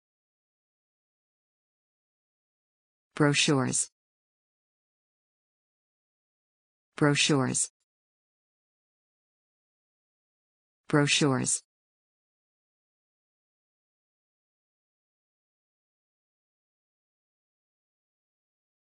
How to say brochures in English
A short video showing how to pronounce brochures.